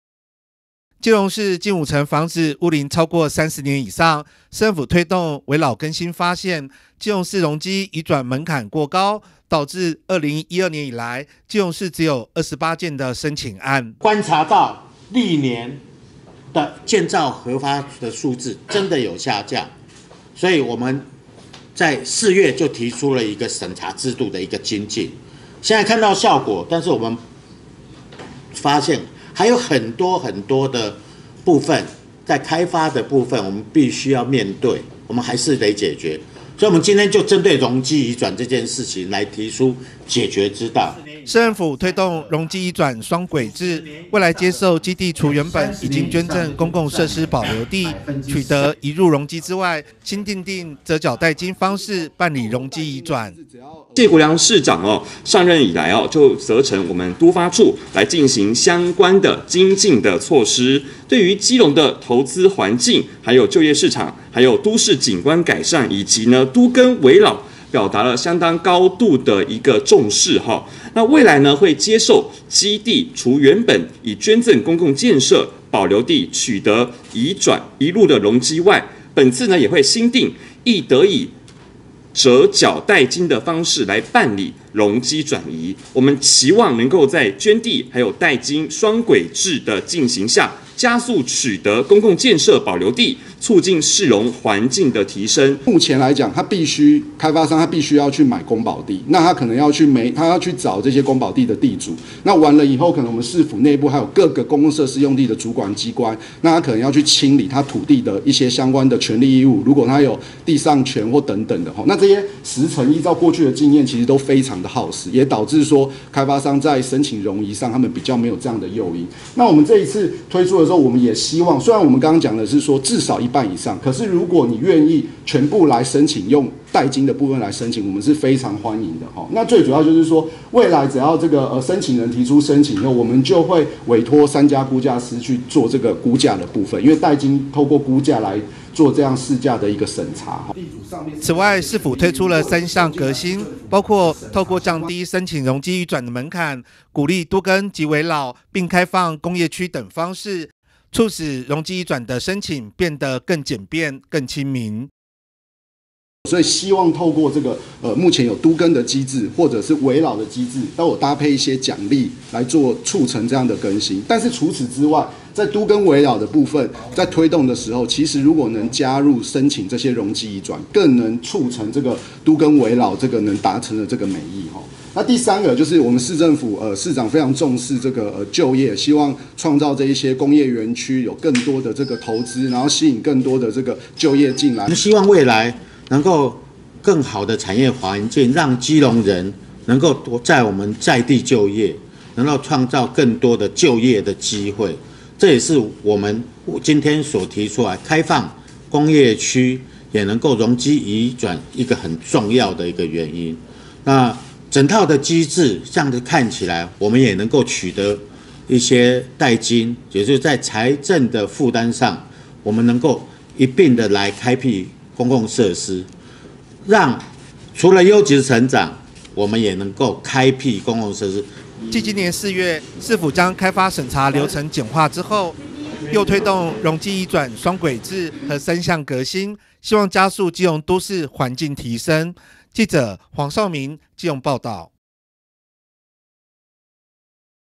改革二部曲｜容積｜基隆都市改革二部曲 容積移轉可以代金取代
(中嘉吉隆新聞 記者黃紹銘/基隆報導) 基隆有近五成房子屋臨超過30年以上，為加速都市景觀改善及都更危老推動，基市政府繼四月調整「都市開發審查流程」，宣布推出「容積移轉雙軌制與三革新」，容積移轉可以折繳代金，在捐地和代金的雙軌進行下，希望有助於老舊建築改建、市容改善。

基隆市近五成房子屋齡30年以上，市政推動危老更新發現，發現基隆市容積移轉的門檻過高，致使2012年以來，基隆市僅有28件申請案。
基隆市政府都發處長 謝孝昆
市政府推出容積移轉雙軌制，未來接受基地除原本已捐贈公共設施保留地，取得移入容積外，新訂定「折繳代金」方式辦理容積移轉。
此外，市府也推出三項革新，透過降低申請容積移轉的門檻、鼓勵都更及危老，並開放工業區等方式，促使容積移轉的申請變得更簡政便民。
繼今年4月市府將開發審查流程的簡化後，市府推動容積移轉雙軌制與3項革新，希望加速基隆都市環境提升。